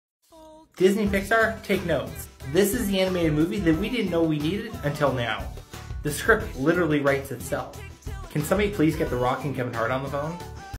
Even Dolphins Need A Wingman!
2 dolphins that are bros and have a bromance?? Just out looking for female dolphins for each other, being each other's wing dolphin. Maybe they are into female dolphins, maybe the adult audience is left wondering if they are also into each other too sometimes. And The Rock and Kevin Hart voicing them?? yes please! It's going to be a hit! You just laid off like 7000 employees. I'm here to save your company and jobs. Let's get a Disney animation division growing that is targeted more for adults. Here's your first movie. I'll even write it for you!